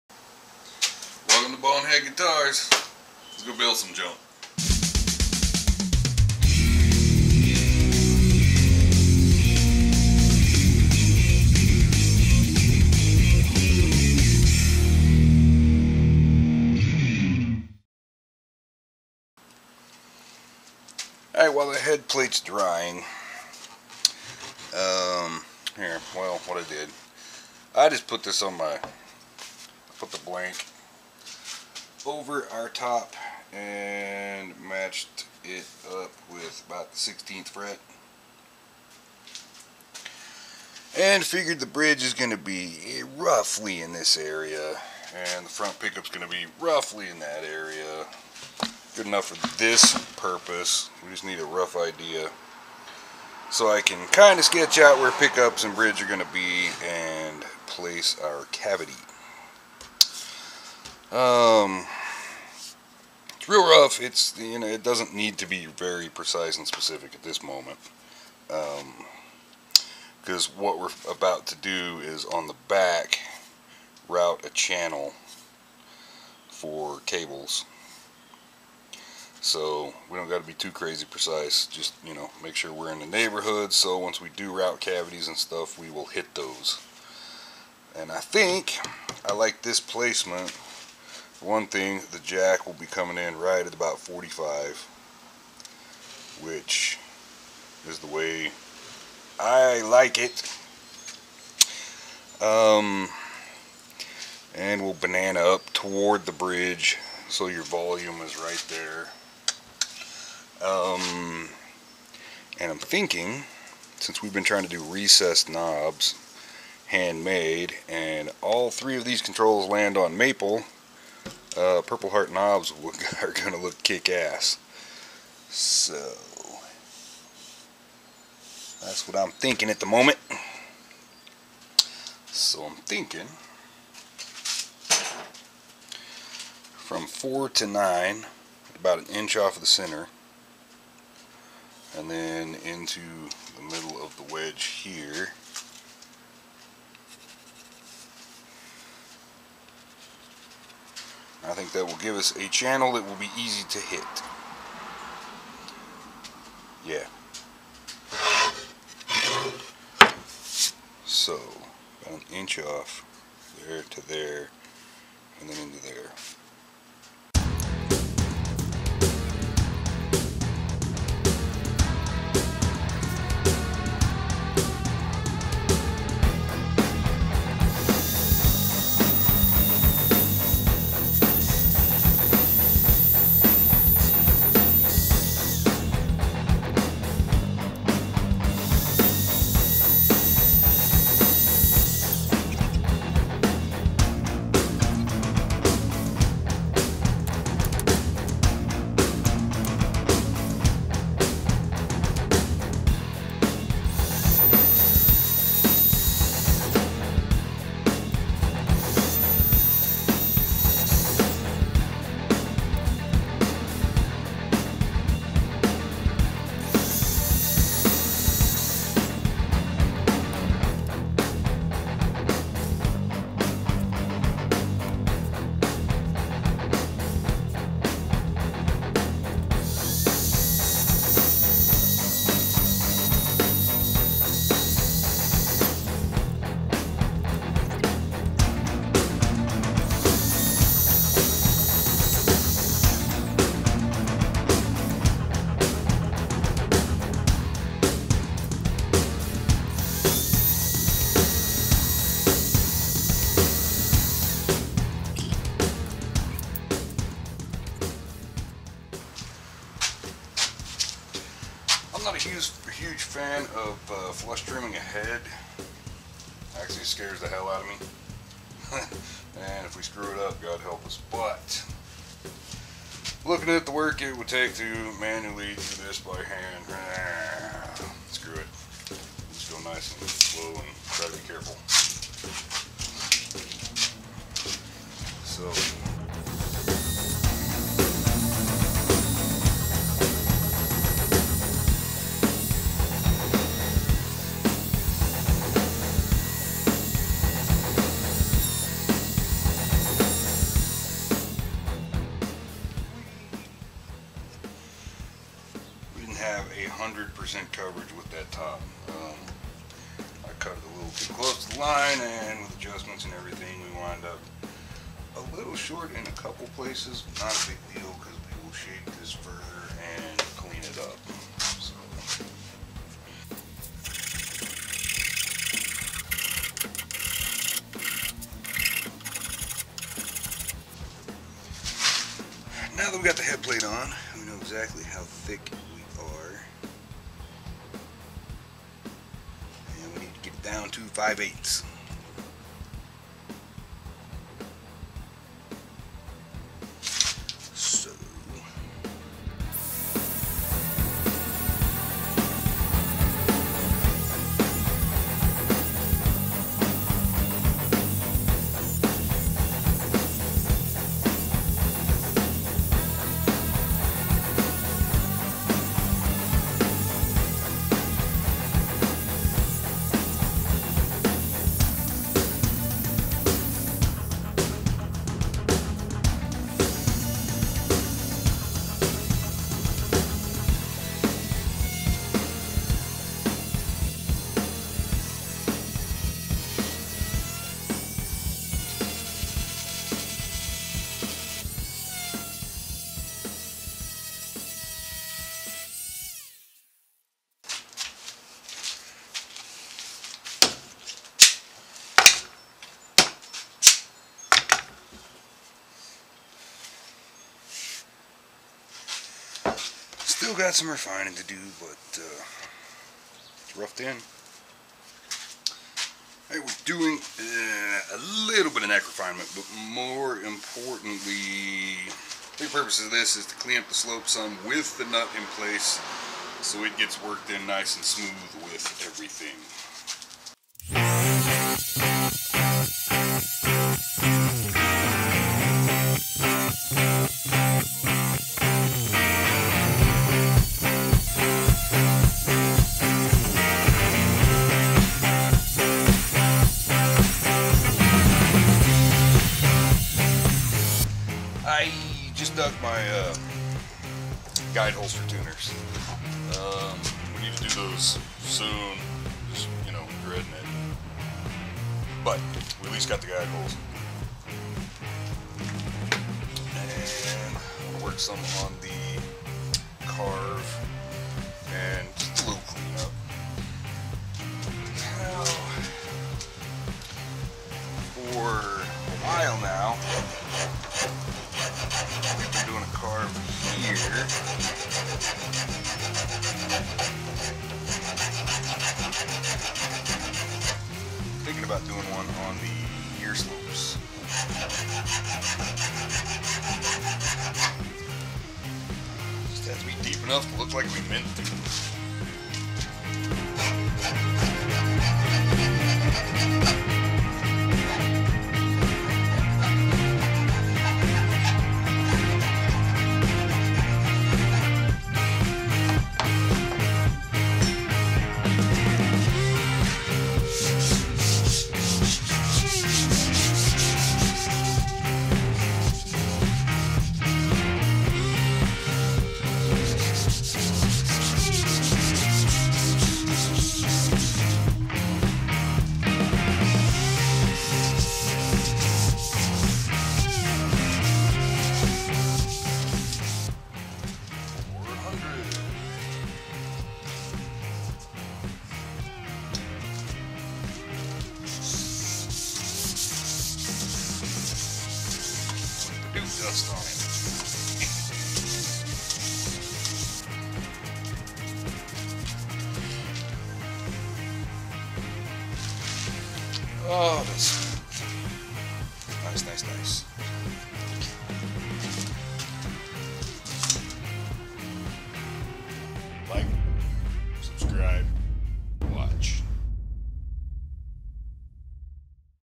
Guitar Build 12.08 - Prepping the Top and Refining the Neck Blank
The scraptastic build that put birdseye maple and purple heart scraps to great use.  This has to be one of the most beautiful things I ever built.

This episode we start getting the top ready for mounting and get closer to finalizing the neck blank.